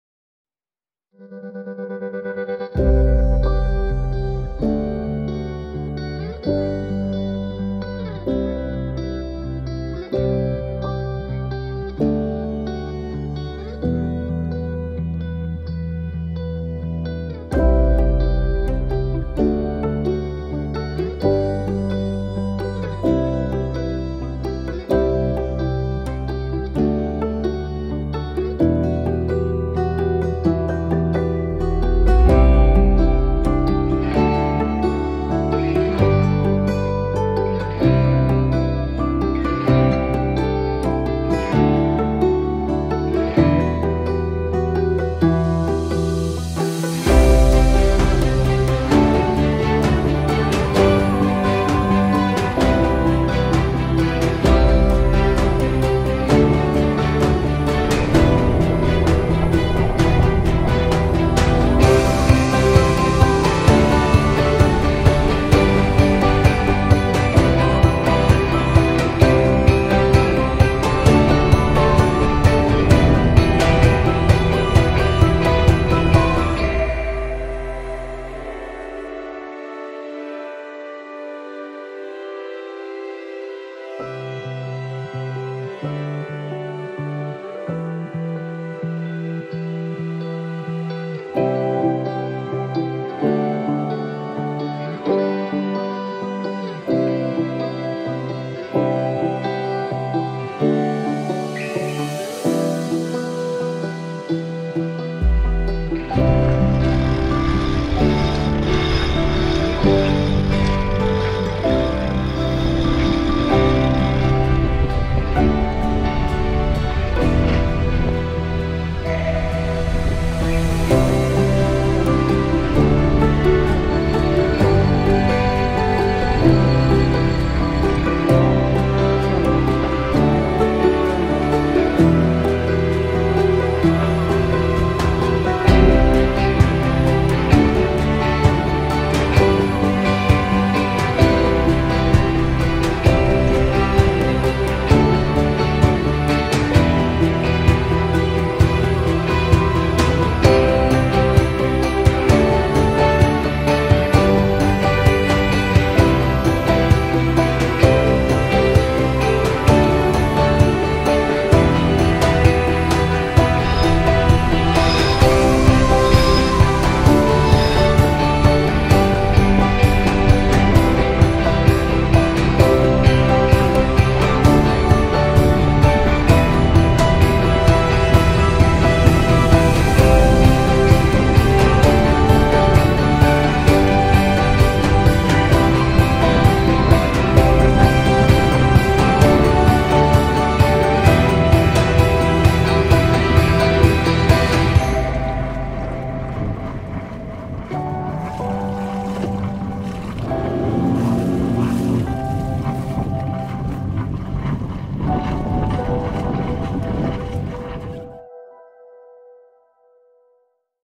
Sweet Swedish October sail
Possibly not the kind of weather you imagine in mid-October in Sweden ... Lovely single handed sailing back home to our marina.